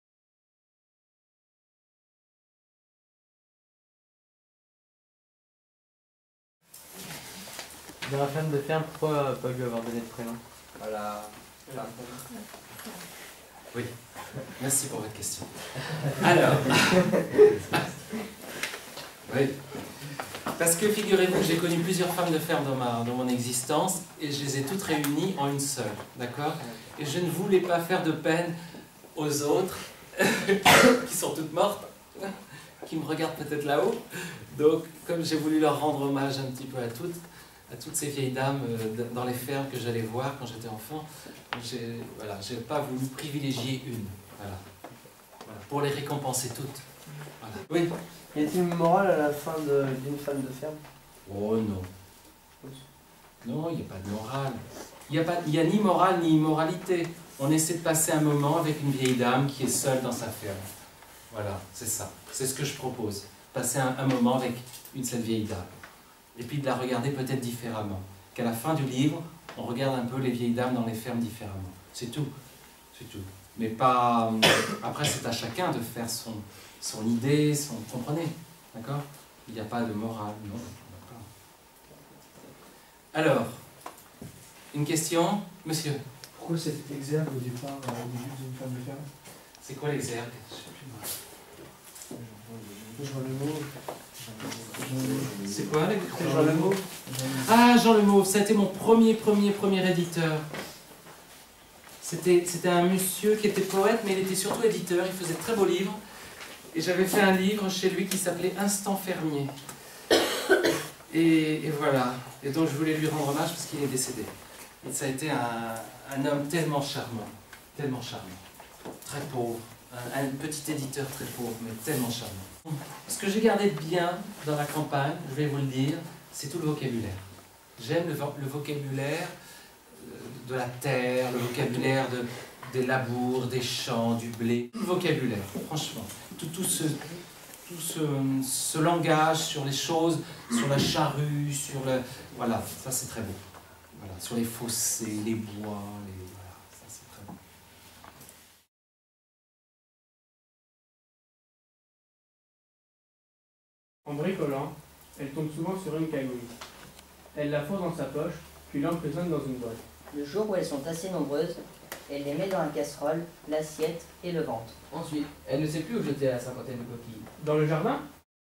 David Dumortier - Une femme de ferme
David Dumortier, poète parle de son livre "Une femme de ferme", éd Cheyne. Film réalisé lors d'une rencontre avec des lycéens dans le cadre du concours Fabriquez un poème. © Centre du livre et de la lecture en Poitou-Charentes - 2013